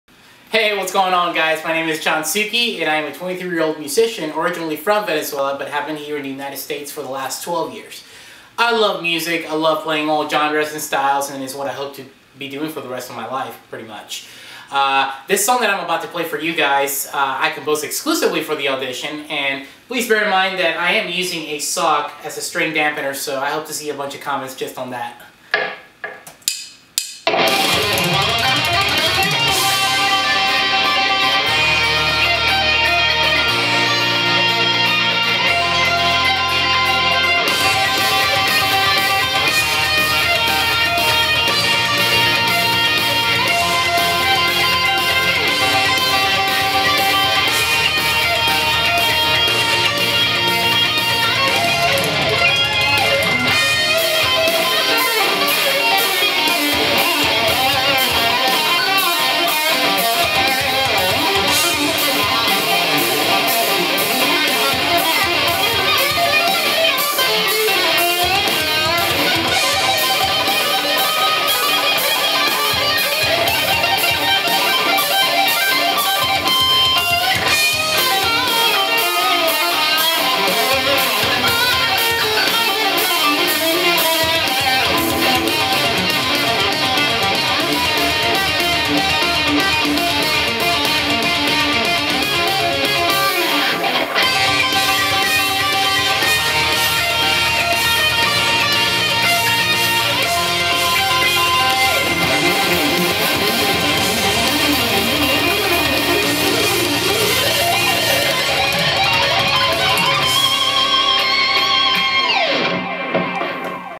Justin Bieber this, Lady Gaga that.... What about guitarists?!
Hey youtubers,

This was my video for this year's AGT Youtube auditions. Unfortunately, I didn't make the Youtube Top 20. Guess I have to start taking singing lessons, put on a Lady Gaga outfit, and get a Bieber cut. haha!

At least my chihuahua appreciated it. maybe? :-P